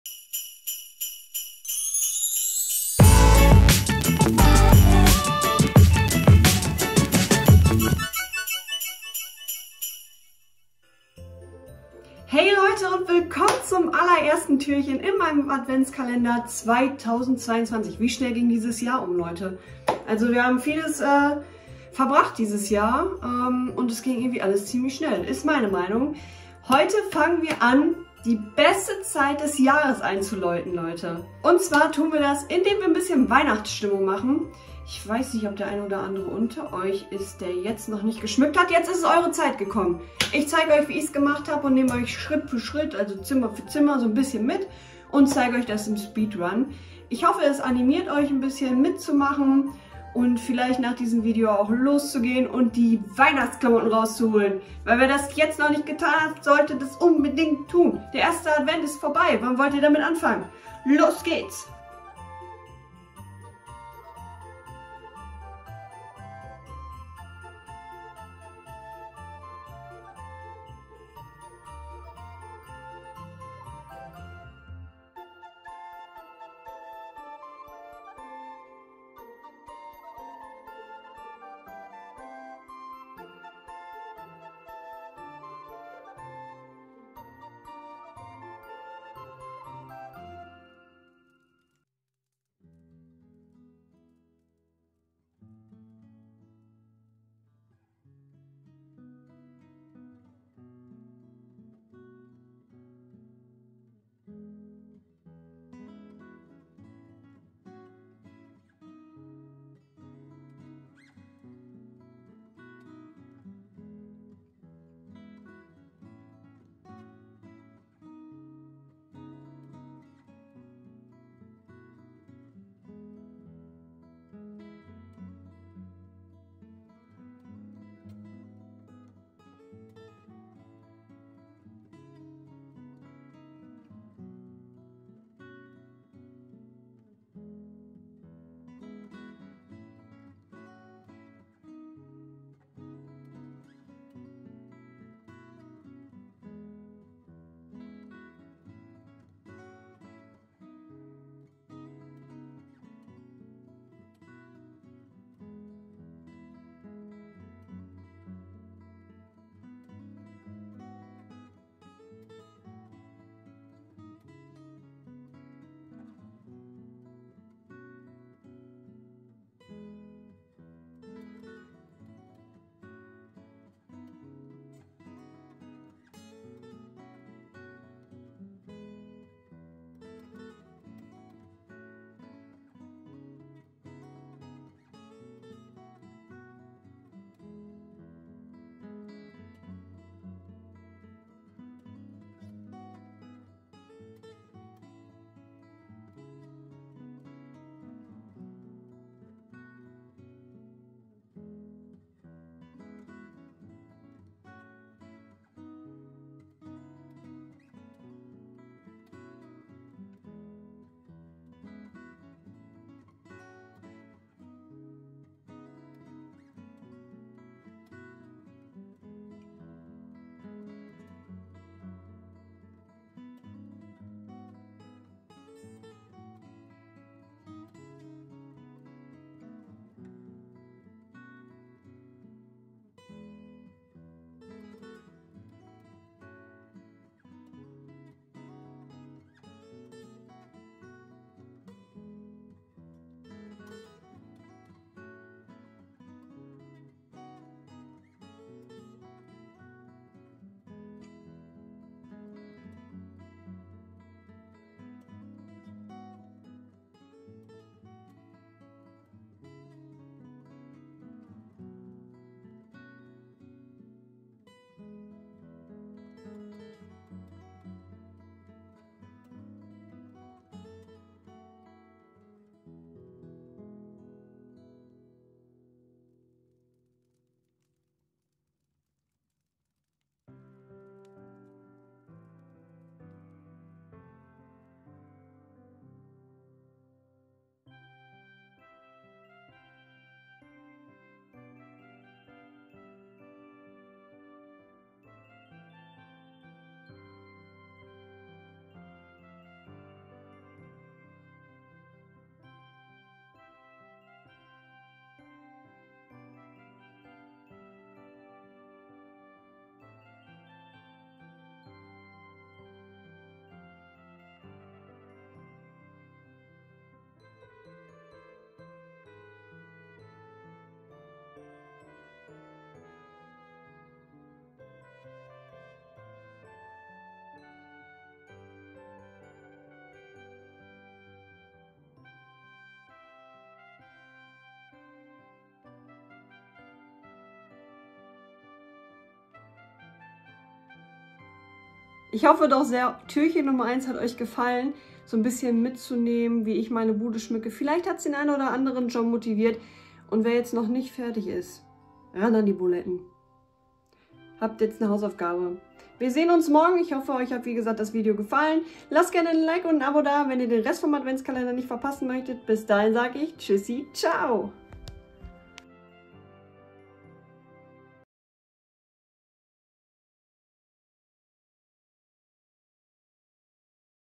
🌟🎄Adventskalender 2022 I Türchen Nr. 1 I Let's decorate with me🎄🌟
Hei Leute und Willkommen zum 1. Adventskalendertürchen 🌟

Es ist an der Zeit, das erste Türchen wird geöffnet und wir dekorieren gemeinsam die Wohnung!

Lasst gerne ein Like und ein Abo da, wenn euch das Ganze gefallen hat und wir sehen und beim nächsten Video wieder.

Design Logo: Kssblog
Design Intro und Outro: Minimal-S